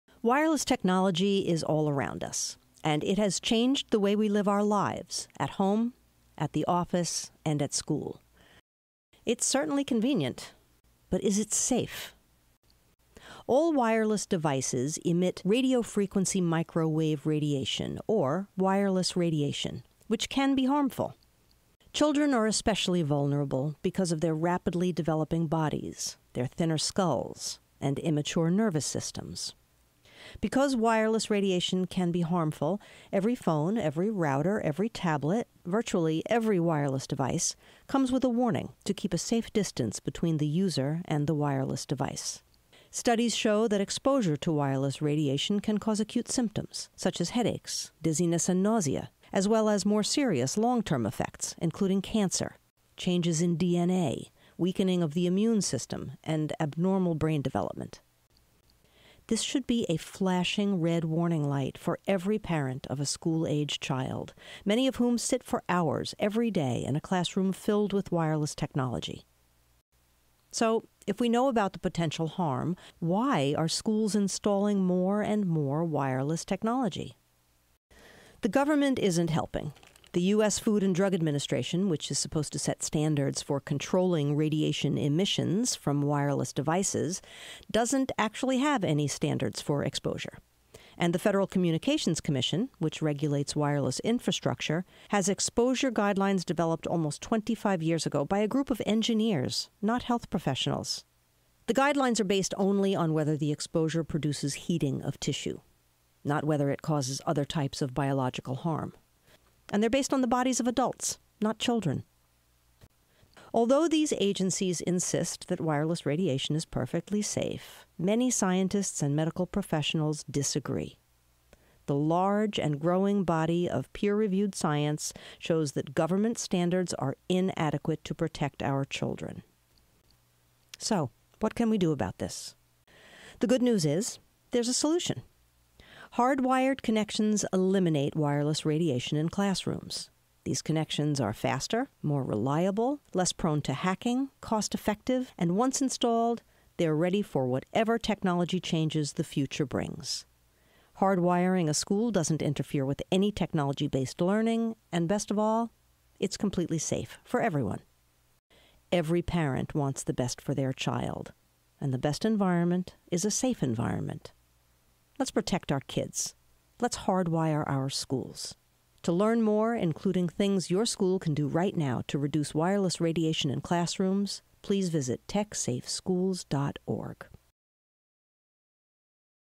Szkoła z bezpieczną technologią
Film jest krótki, ale niezwykle treściwy - odpowiada na następujące pytania:

- Jakie potencjalne niebezpieczeństwo kryje się za nowoczesną technologią?
- Czy współczesna szkoła może być zarówno nowoczesna, jak i bezpieczna?

Film znalazłam na kanale YT GrassrootsEnvEd należącym do organizacji Grassroots Environmental Education Organizacja ta stanowi, jak jej angielska nazwa wskazuje, oddolną inicjatywę na rzecz edukacji środowiskowej. Zajmuje się kwestiami takimi jak czysta woda, czyste powietrze, bezpieczna żywność, a także - wpływ promieniowania elekromagnetycznego na zdrowie człowieka, a w szczególności - na zdrowie dzieci. Skontaktowałam się z jej przedstawicielem, proponując przetłumaczenie filmu na język polski i zaprezentowanie go na moich stronach internetowych - dla polskich rodziców, nauczycieli, dyrektorów szkół. Spotkałam się z pozytywną odpowiedzią. Dowiedziałam się, że film powstał dzięki Americans for Responsible Technology - tu znajduje się przekierowanie do pierwotnego źródła

Twórcy Grassroots Environmental Education , jak również TechSafeSchools współpracują z naukowcami i specjalistami - praktykami. Ich wypowiedzi zaprezentowane są w filmach na wskazanych stornach internetowych, w tym na kanale YT GrassrootsEnvEd. Są to bardzo wartościowe i wiarygodne źródła informacji na temat zagrożeń zdrowotnych powodowanych przez promieniowanie elektromagnetyczne pochodzące ze sztucznych źródeł - gorąco polecam zapoznanie się z materiałem.